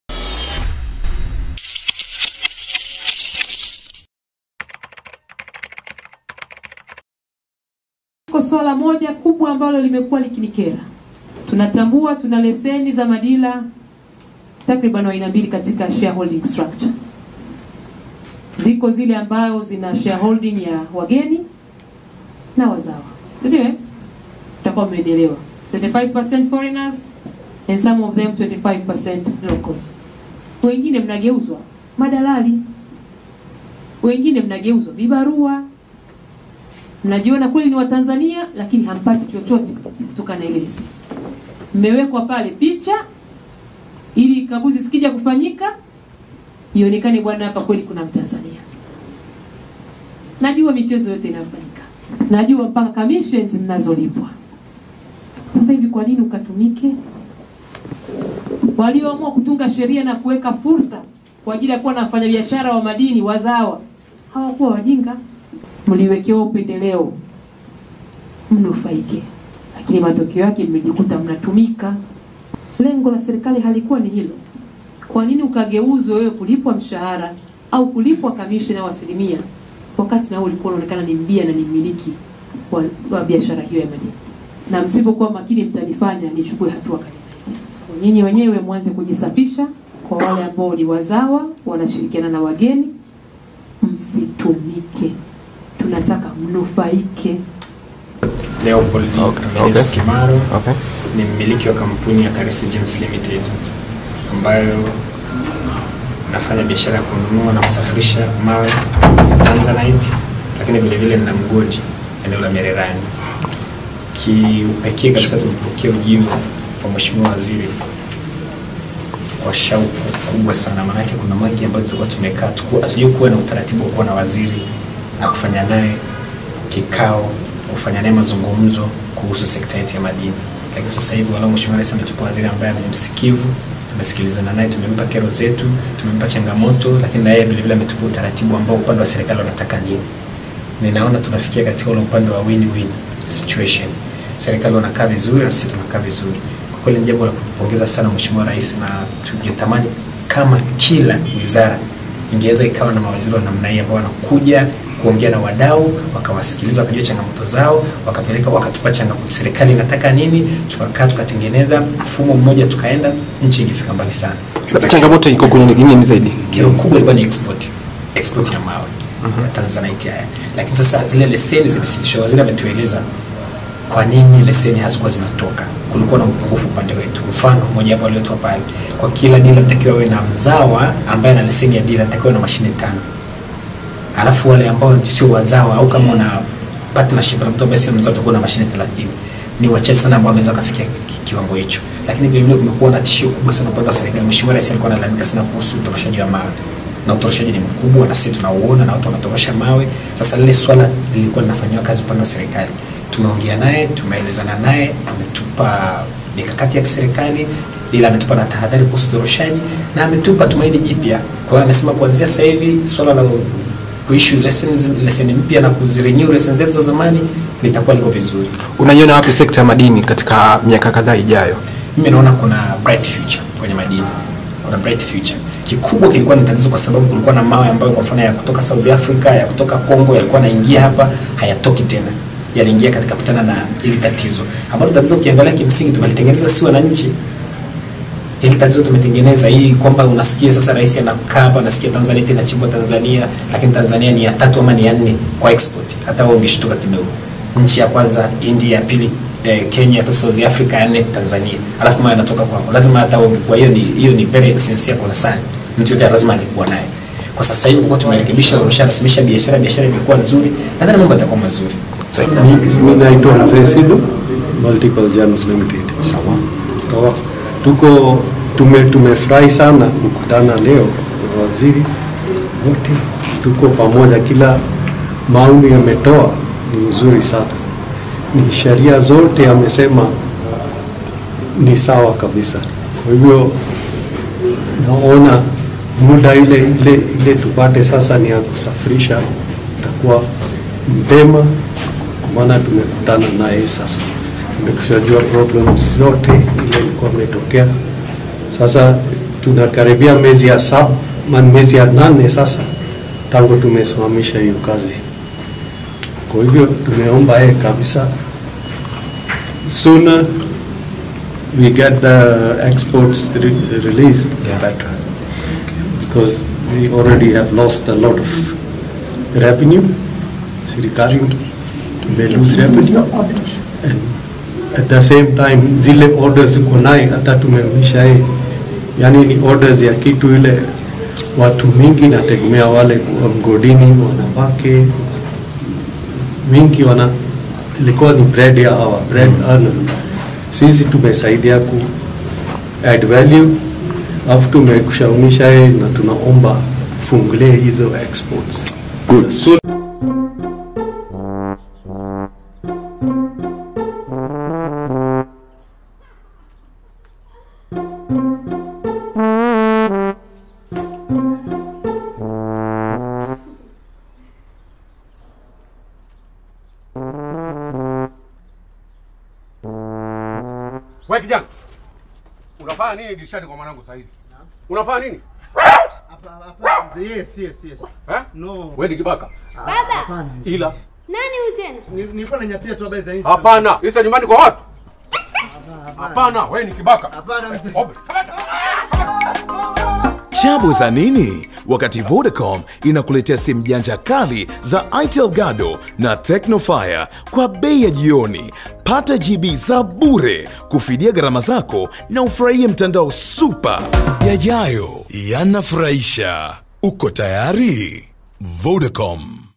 WAZIRI KAIRUKI- "Najua Michezo Yote / Kwanini Ukageuzwa wakala"
WAZIRI -  "Najua Michezo Yote / Kwanini Ukageuzwe?"

Waziri wa Madini, Angela Kairuki amewataka watanzania wanaojishughulisha na Biashara ya Madini kukataa kuwa madalali wa wauzaji na wanunuaji wa madini kutoka nje ya nchi.